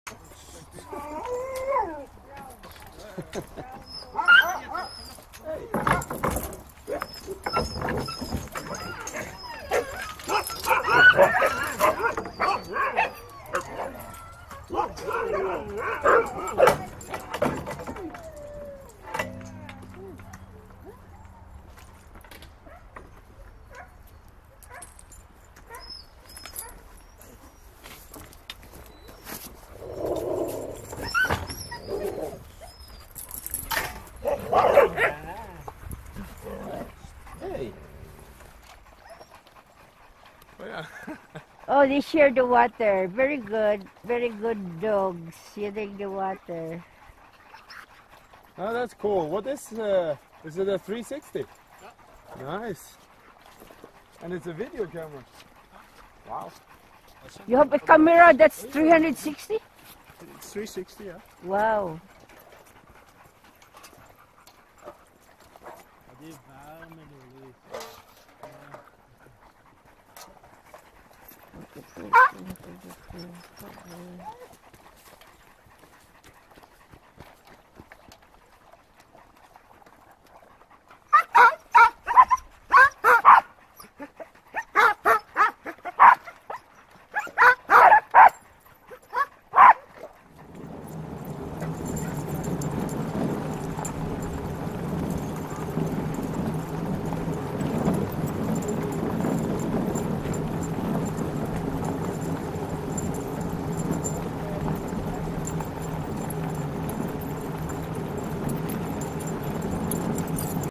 svalbard husky ride in Longyearbyen
360 degree movie of my dog sledge experience in Svalbard.